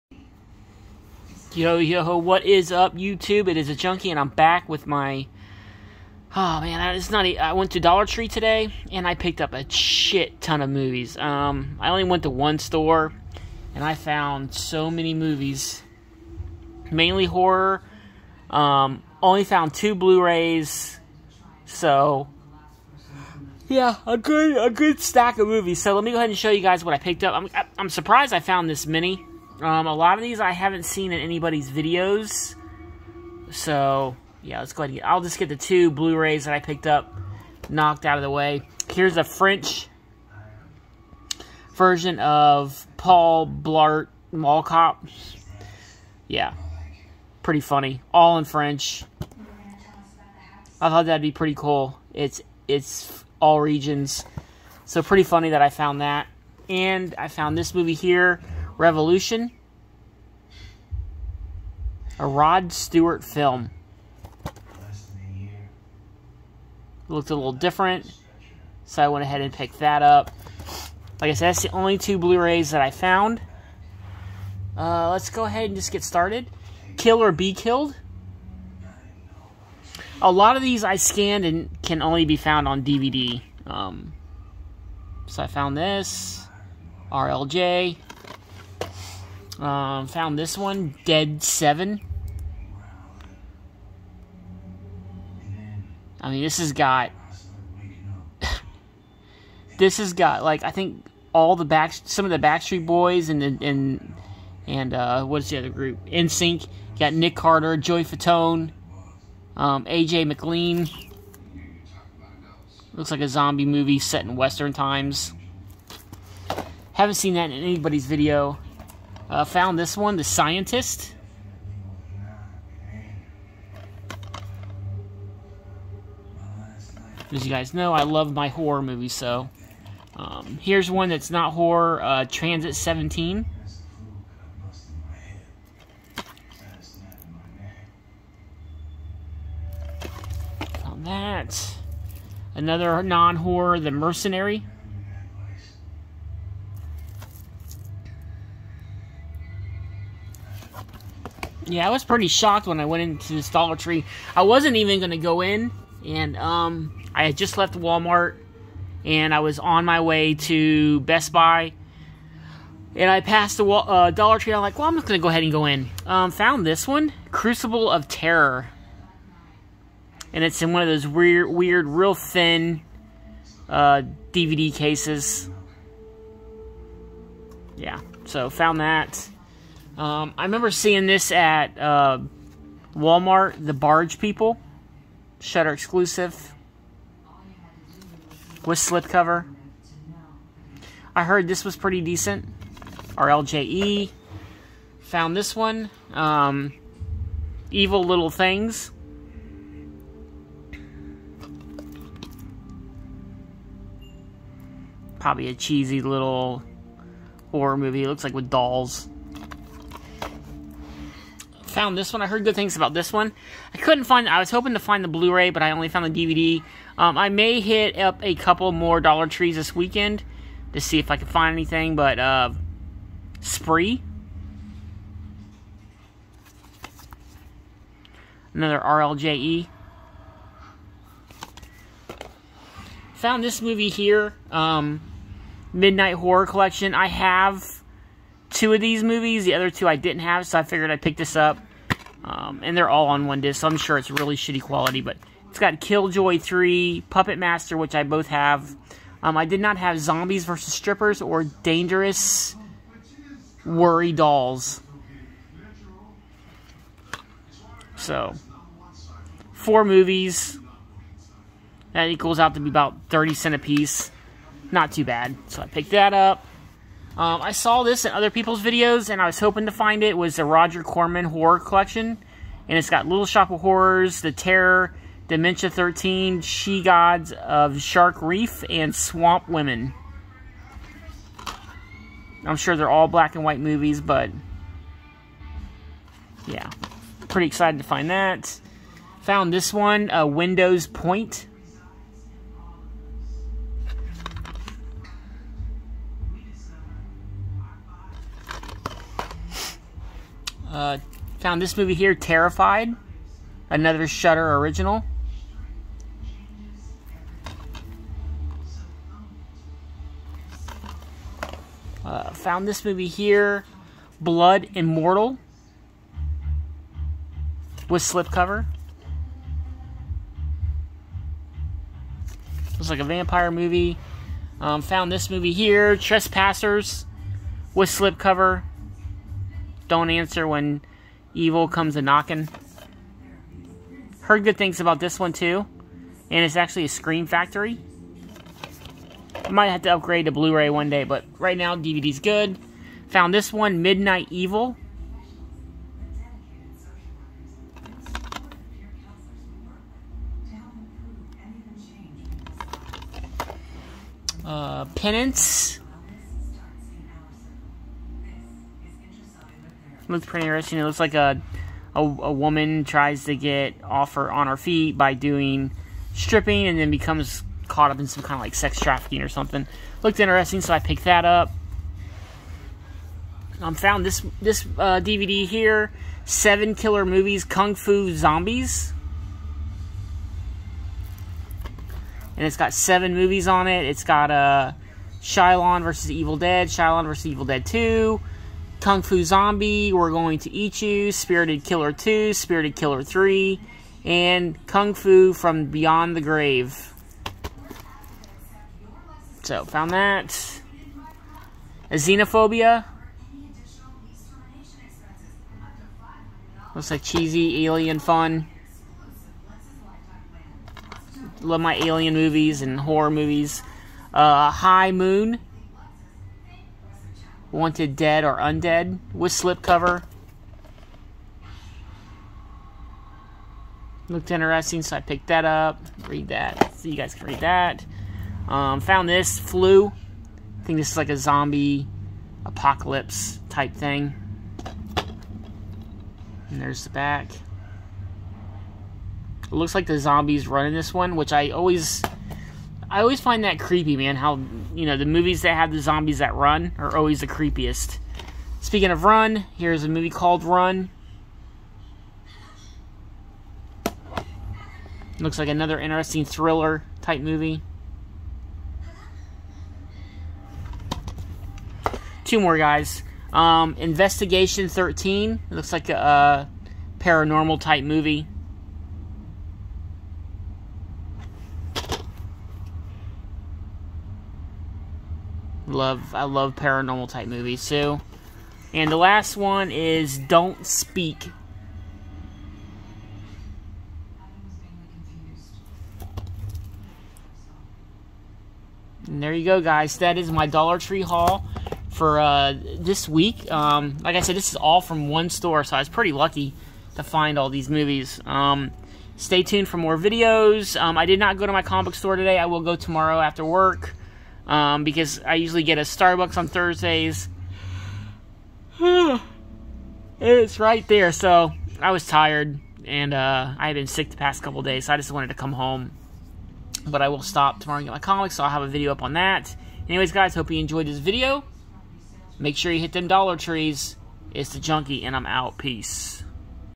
DOLLAR TREE MOVIE HAUL!! 03/09/22
In this video I went dvd and blu ray hunting at Dollar Tree today. What did I pick up?